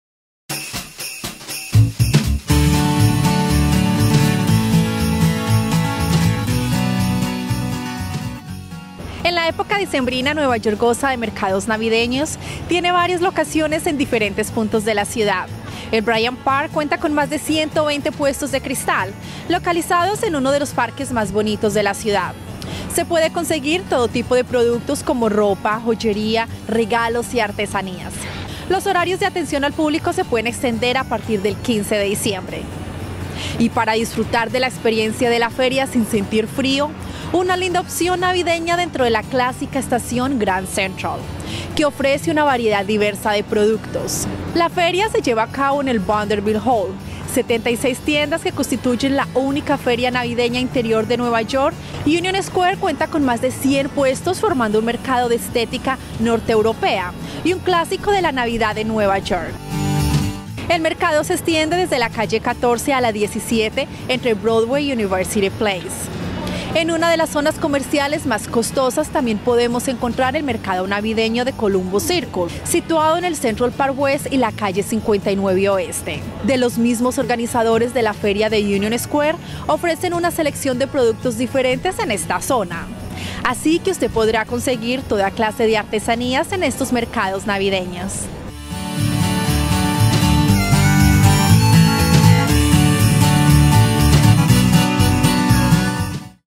Mercados Navideños New York /
En la época decembrina Nueva York goza de mercados navideños;  tiene varios locaciones en diferentes puntos de la ciudad
El Bryant Park, Unions Square, Grand Central y Columbus Circle.
Na época decembrina Nova York goza de mercados navideños; tem vários locaciones em diferentes pontos da cidade O Bryant Park, Unions Square, Grand Central e Columbus Circle.